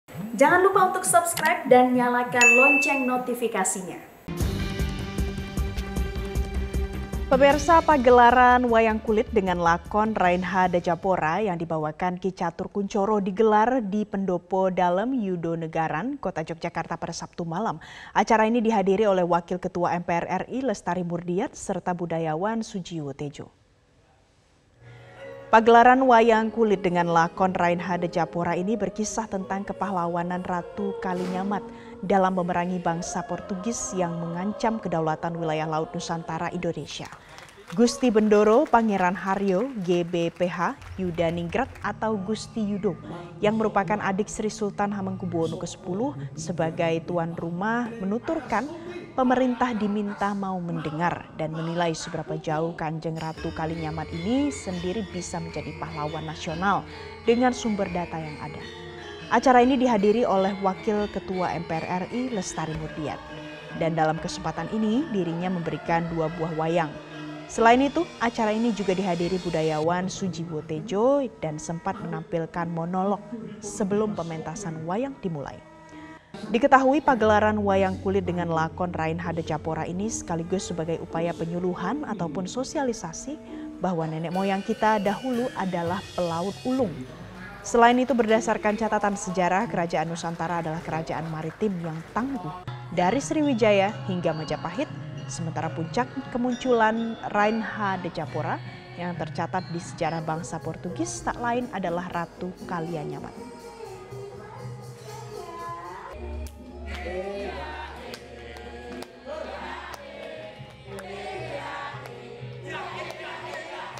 Pagelaran Wayang Kulit Lakon Rainha De Japora Kisahkan Perjuangan Ratu Kalinyamat
Pagelaran wayang kulit dengan lakon Rainha De Japora yang dibawakan Ki Catur Kuncoro digelar di Pendopo Dalem Yudonegaran, Yogyakarta pada Sabtu (4/6/2022) malam. Acara ini dihadiri Wakil Ketua MPR RI Lestari Moerdijat serta budayawan Sujiwo Tejo.